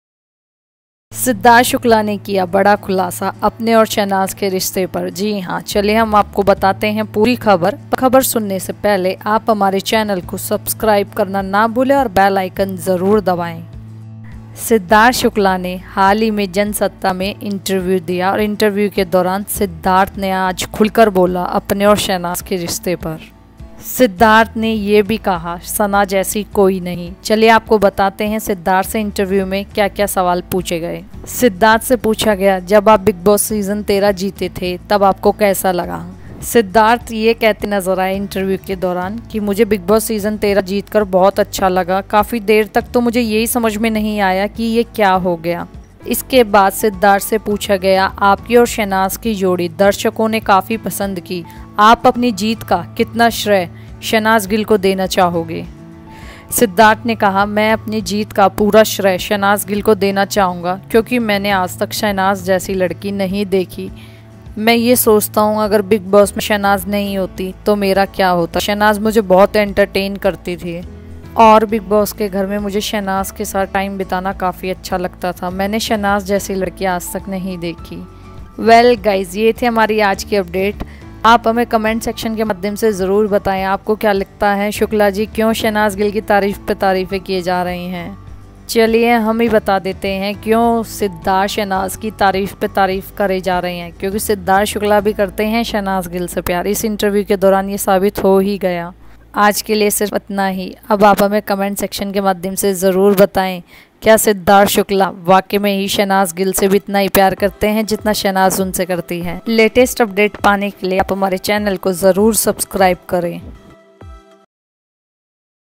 Siddharth Shukla Interview: Siddharth ने कहा Shehnaaz जैसी कोई नहीं, वो मेरे लिए Family से भी बढ़कर
Jansatta Express Interviews Siddharth Shukla and asked many questions related to Shehnaz Gill.

Note: All the images/pictures shown in the video belong to the respected owners. I am no the owner of any pictures showed in the video.